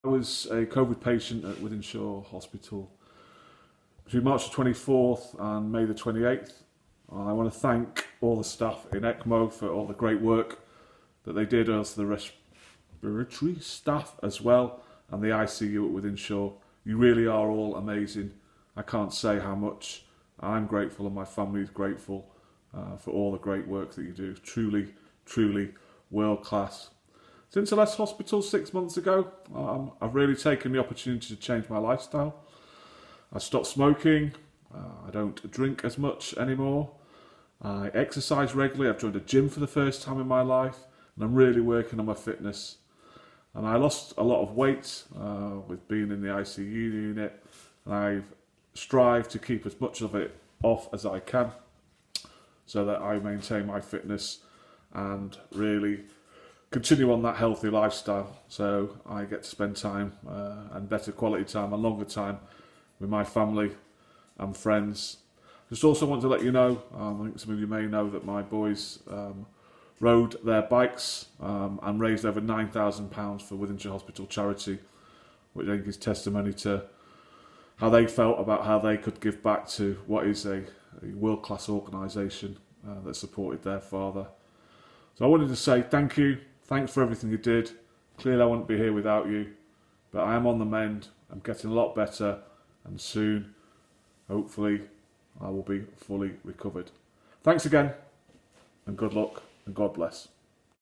Covid survivor Daimon Keith thanks Wythenshawe Hospital staff for saving his life.
Daimon Keith is a UK Covid survivor thanks to the wonderful efforts of Wythenshawe Hospital staff in Manchester. To show just how much the family appreciated their help in saving his life, Daimon's family raised more than £9,000 for Wythenshawe Hospital Charity.